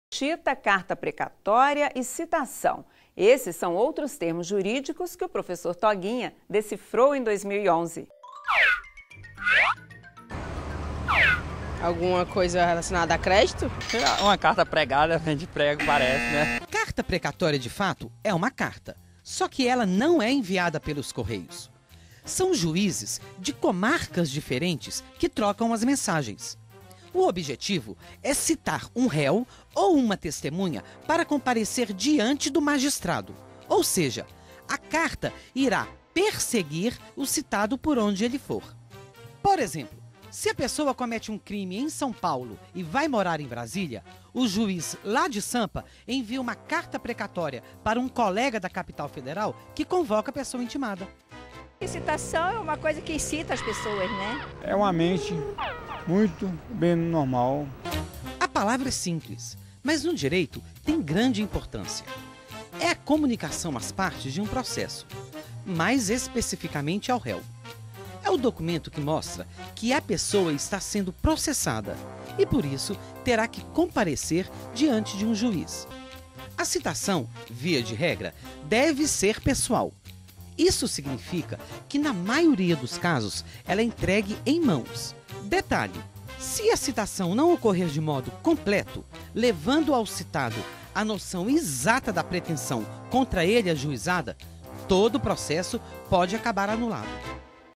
Prof. Toguinha - carta precatória e citação
Veja o que significa carta precatória e citação